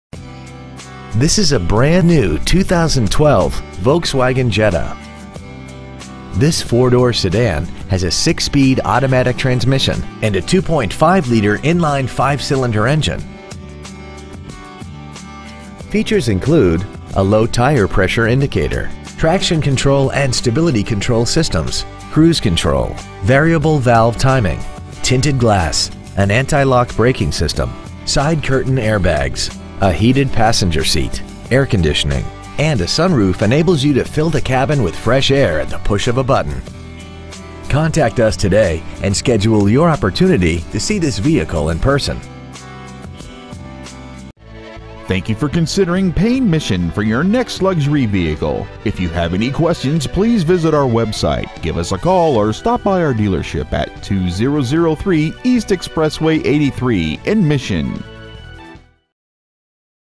2012 Volkswagen Jetta Monterrey MEX
Year : 2012
 Make : Volkswagen
 Model : Jetta
 Engine : 2.5L 5 cyls
 Trans . : Automatic 6-Speed
 Exterior : Gray
 Miles : 10
 Interior : Titan Black w/Titan Black V-Tex Leatherette Seat T
 Stock : M351747

 2003 E. Expressway 83